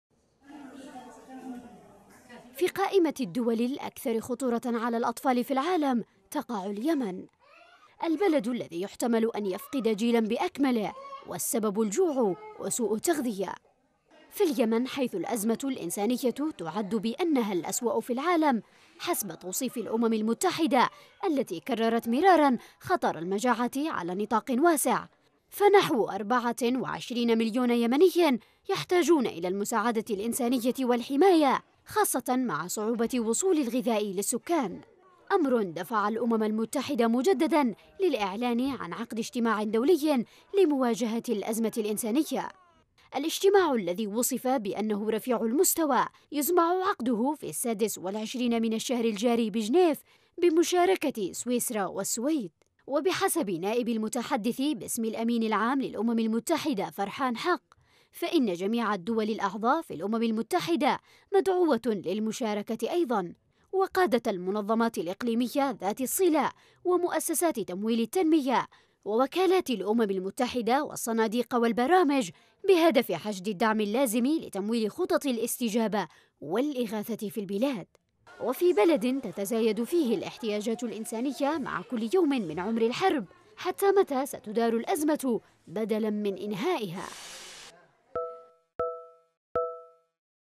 منظمة معنية بحقوق الطفل تدرج اليمن في قائمة الدول الأكثر خطورة على الأطفال | تقرير: شروق القاسمي
أعلنت الأمم المتحدة عقد اجتماع دولي رفيع المستوى الشهر الجاري في جنيف بخصوص مواجهة الأزمة الإنسانية في اليمن.
يأتي هذا بالتزامن مع إعلان منظمة أنقذوا الأطفال المعنية بحماية حقوق الطفل، إدراج اليمن في قائمة الدول الأكثر خطورة على الأطفال في العالم الى جانب سوريا والعراق...